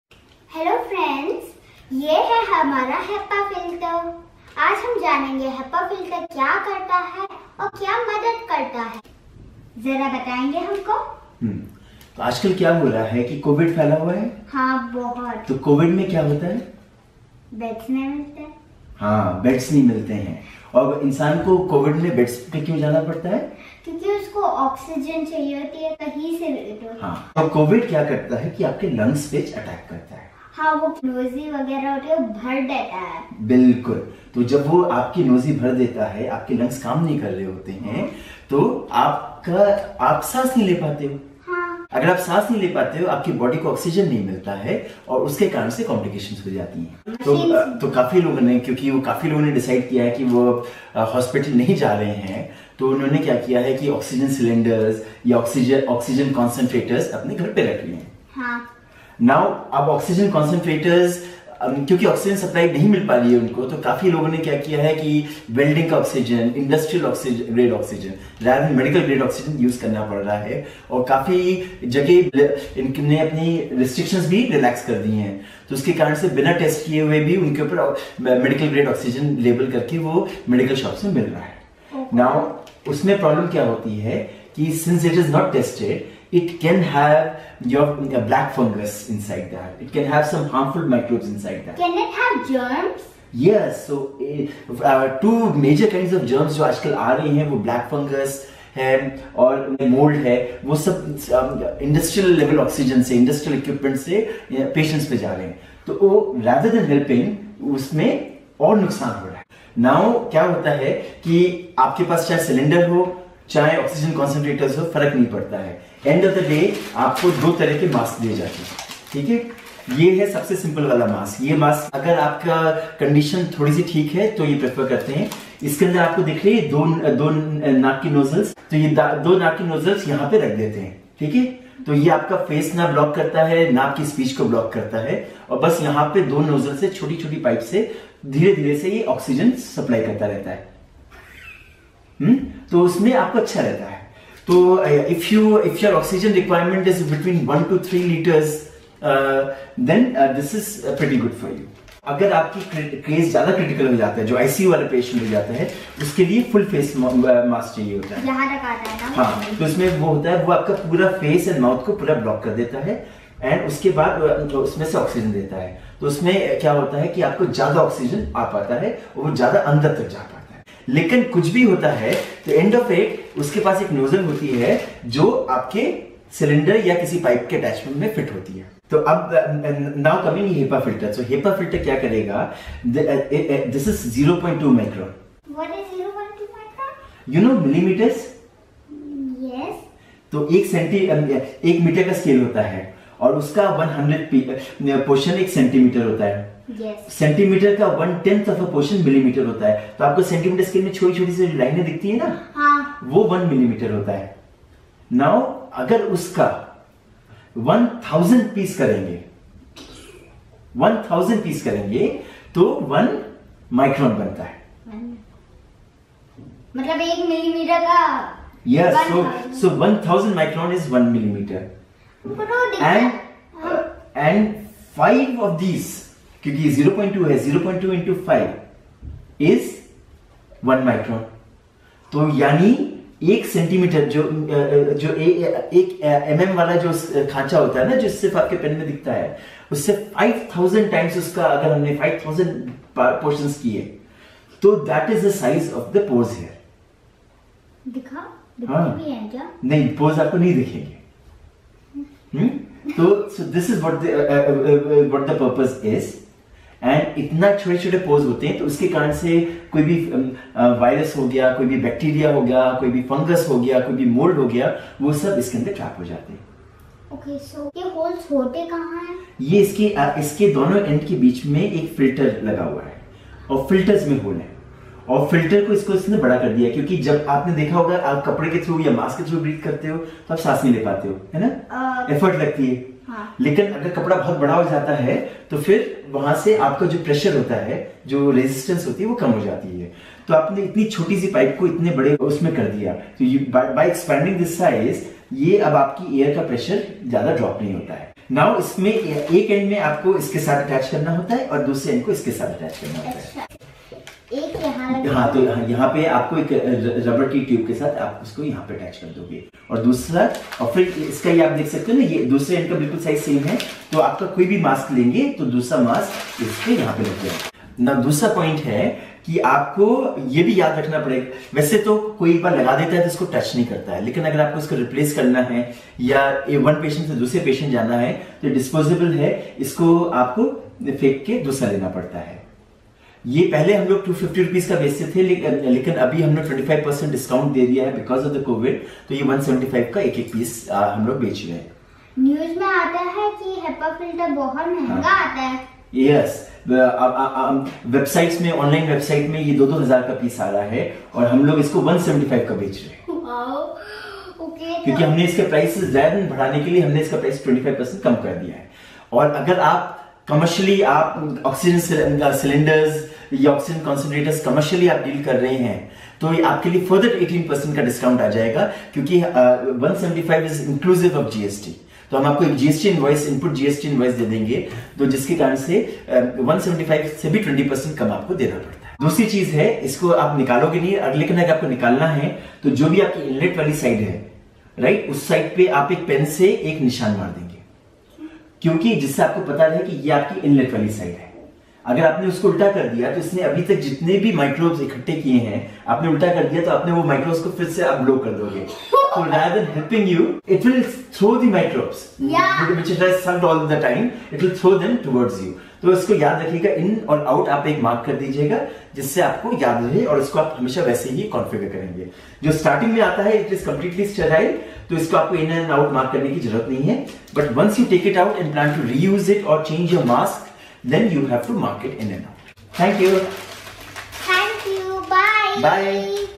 HEPA Filter for Oxygen Cylinders and Concentrators | 0.2 micron | ARISHTAM INDIA
At Arishtam we have a variety of tubing, fittings, filters that can be used to make a oxygen delivery system at home. We have reduced the price of HEPA filters by 25% to 175/- so that healthcare and COVID management is easy and affordable for all.